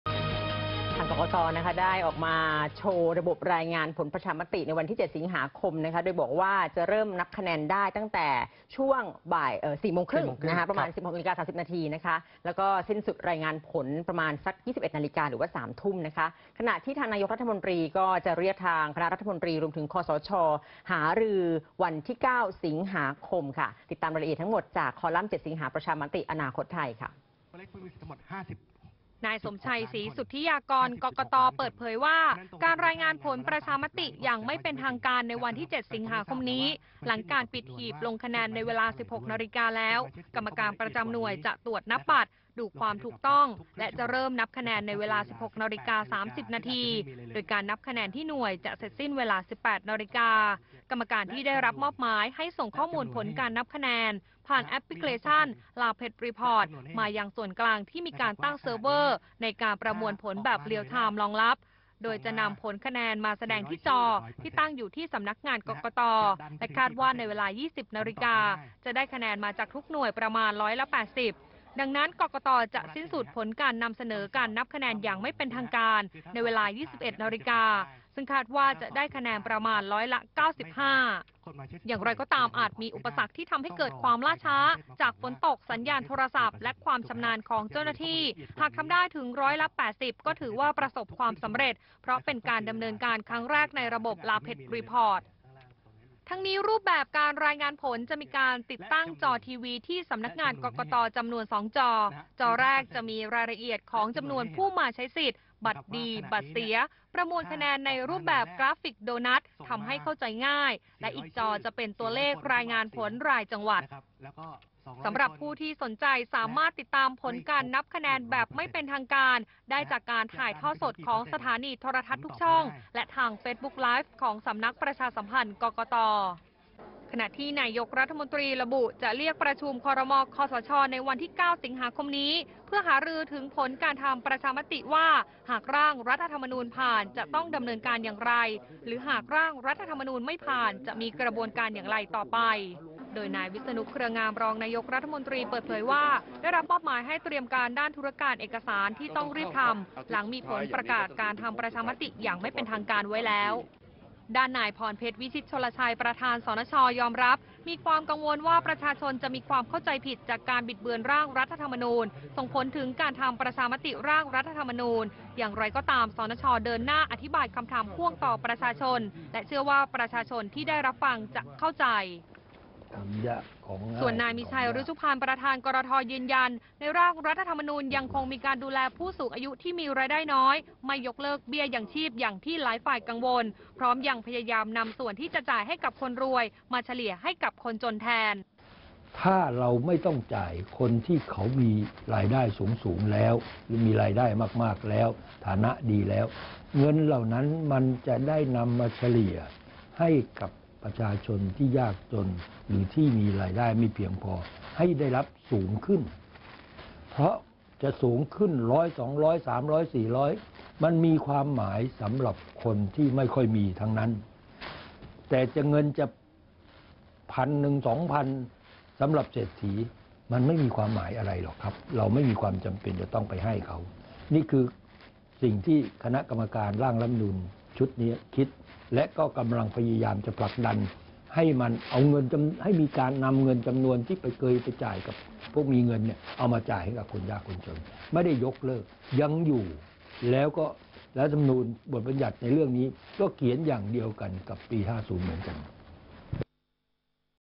7 สิงหา ประชามติ : กกต.โชว์ระบบรายงานผลประชามติ
กกต.โชว์ระบบรายงานผลประชามติ 7 สิงหาคม เริ่มนับคะแนนได้ 16.30 น. สิ้นสุดรายงานผล 21.00 น. ขณะที่นายกรัฐมนตรีจะเรียก ครม.-คสช. หารือ 9 สิงหาคม